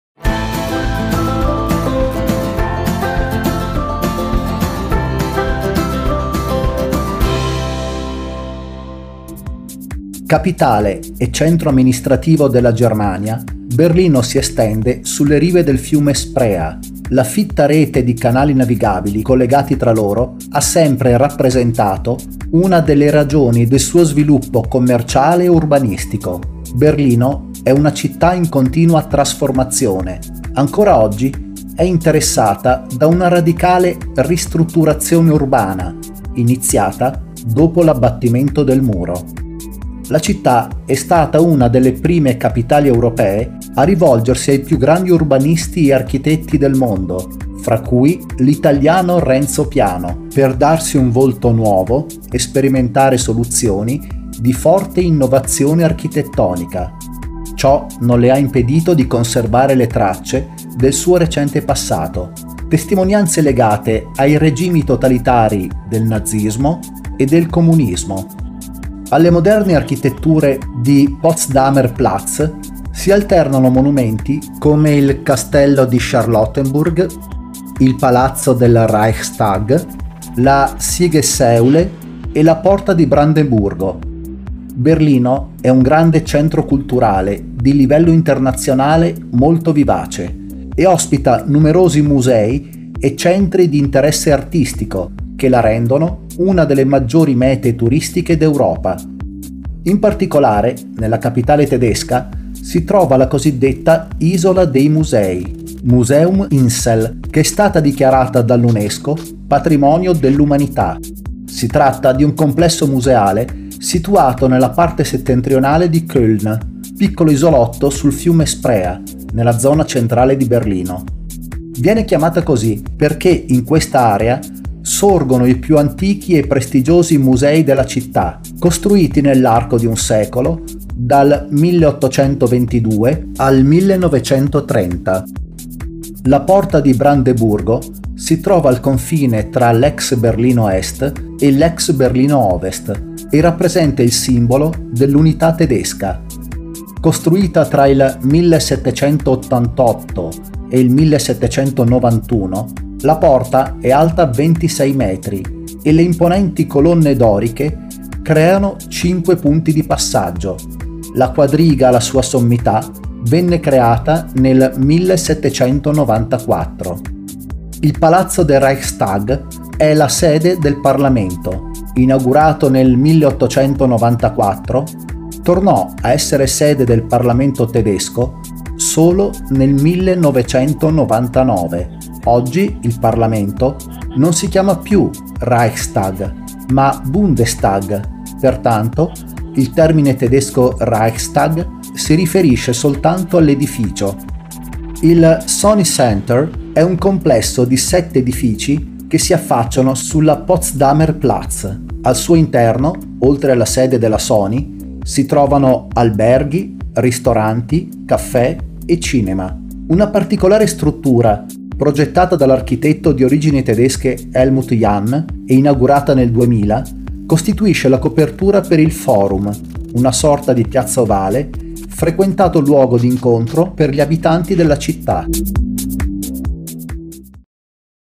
In giro per... Berlino
La città di Berlino è ricchissima di monumenti e musei. Un luogo particolarmente interessante è l’Isola dei Musei, che ospita – come lo stesso nome suggerisce – cinque musei tra i più importanti del mondo: l’Altes Museum, il Neues Museum, l’Alte Nationalgalerie, il Bode-Museum e il Pergamonmuseum.